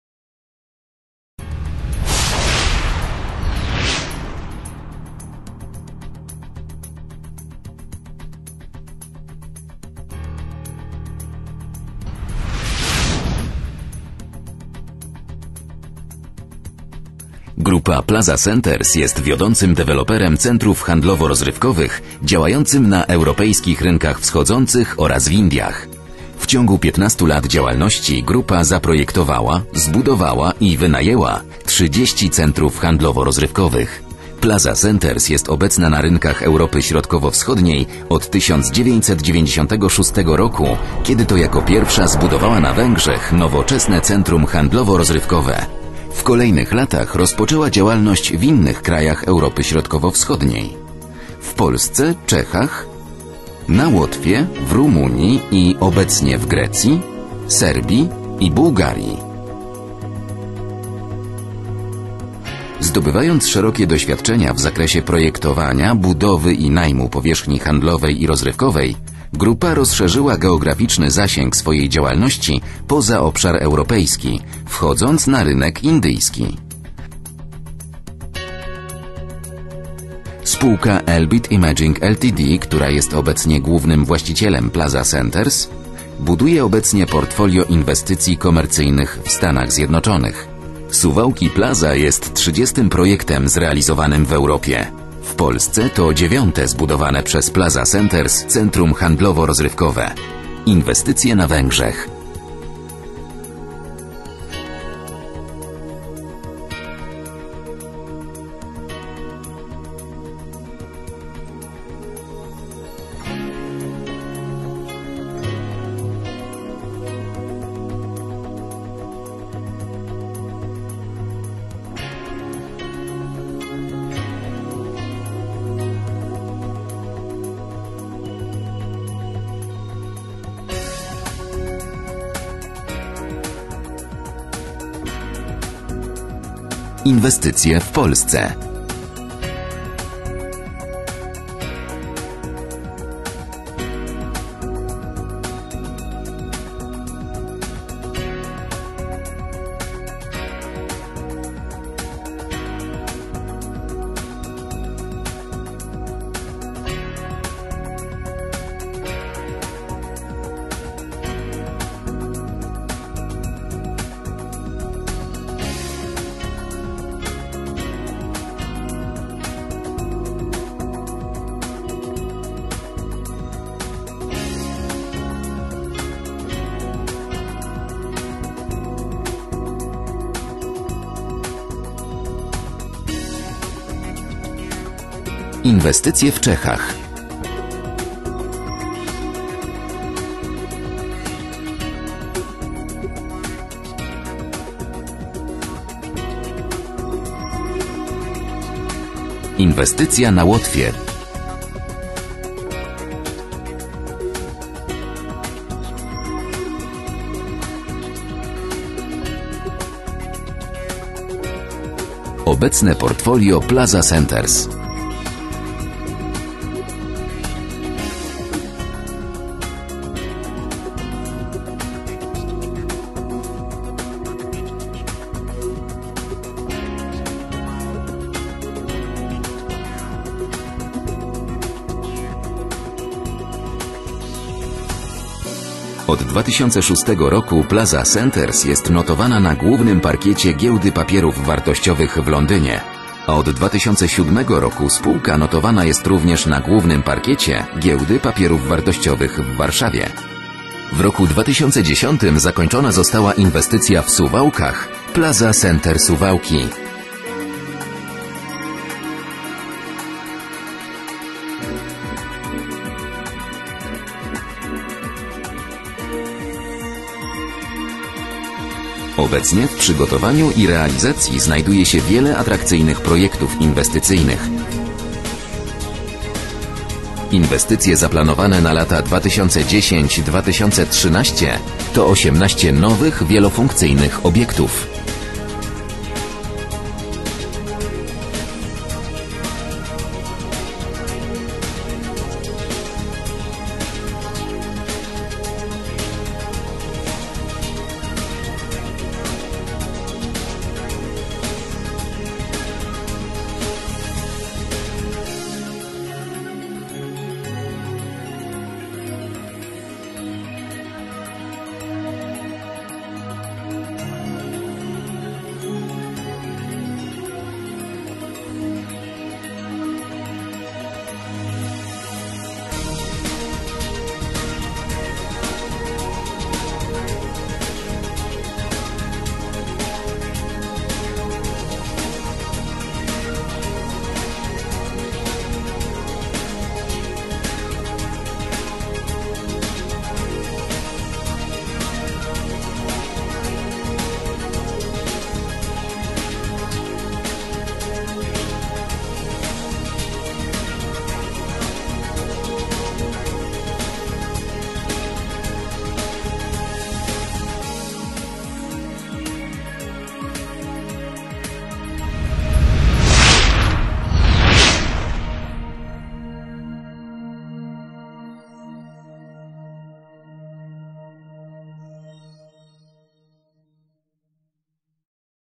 Plaza Centers
Korporacyjny film z okazji otwarcia 30 centrum Plaza Centers w Europie.